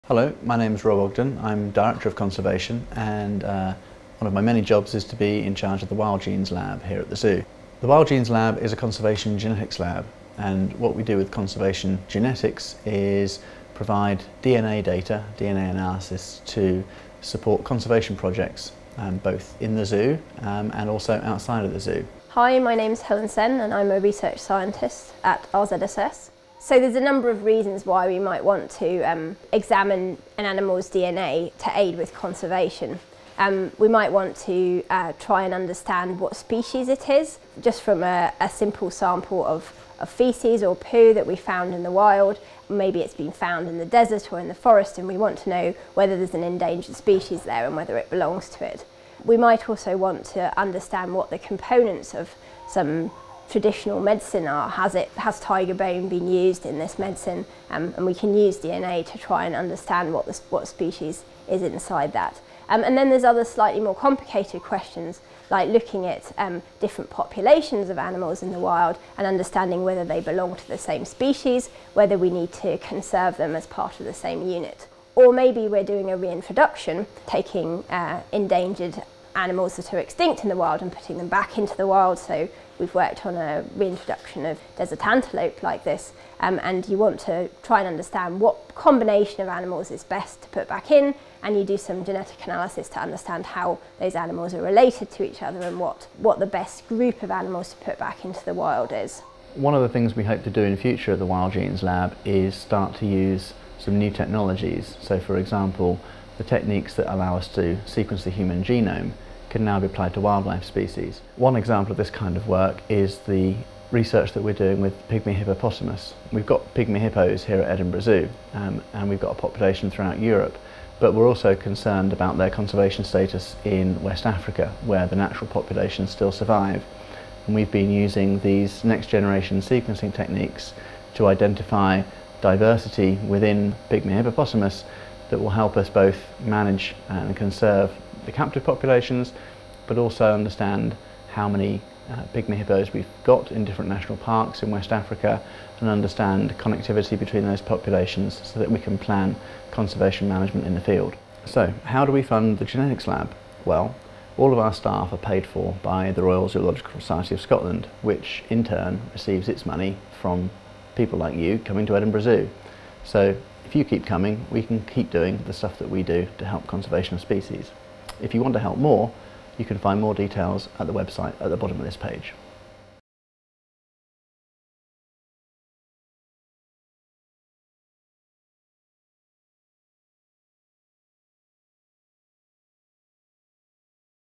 WildGenes Laboratory - RZSS Conservation - Great days out for a good cause
The RZSS Wildgenes Laboratory is based at Edinburgh Zoo. It was established in 2010 and houses research staff and technicians that provide state-of-the-art DNA analysis in support of conservation projects of national and international interest.
From studies of ecology and evolution to applications in zoo breeding programmes, genetic information is advancing our knowledge of the world's flora and fauna and improving our ability to conserve it.